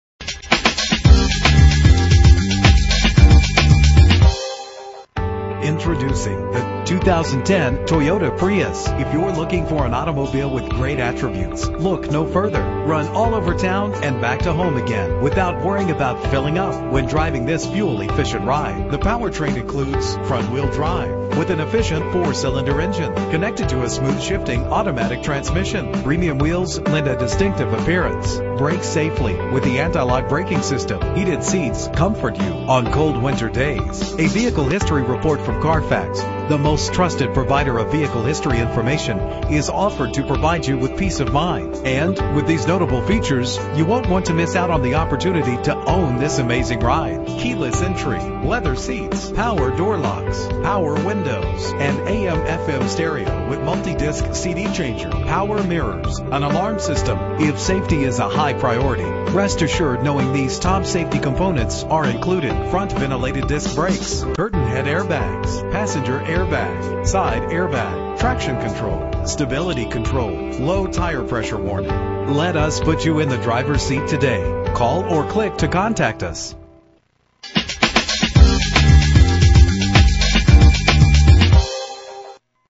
2010 Toyota Prius - Daphne AL
Year: 2010
Make: Toyota
Model: Prius
Trim: V
Engine: 1.8 liter inline 4 cylinder 16 valve
Transmission: Automatic CVT
Color: White
Mileage: 30108
Address:
29732 Frederick Blvd.